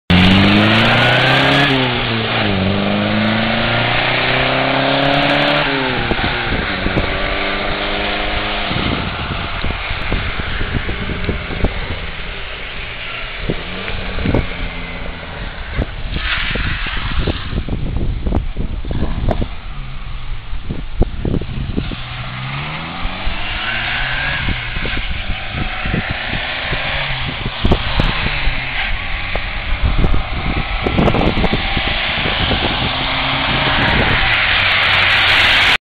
Mini 1000 com novo motor
A testar o novo motor. Ainda em afinação